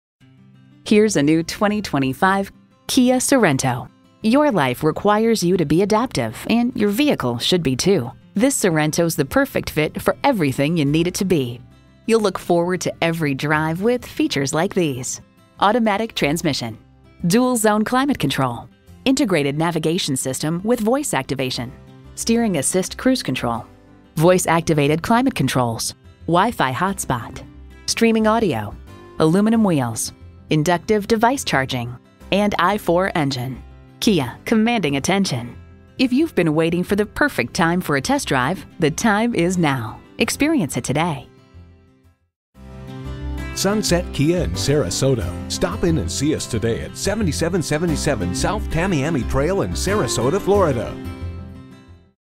New 2025 Kia Sorento Sarasota, FL
Year: 2025
Make: Kia
Model: Sorento
Trim: S
Engine: Regular Unleaded I-4 2.5 L/152
Transmission: Automatic
Color: Wolf Gray
Interior: Grey
Stock #: K11304
VIN: 5XYRL4JC8SG326155

Address:
7777 S Tamiami Trail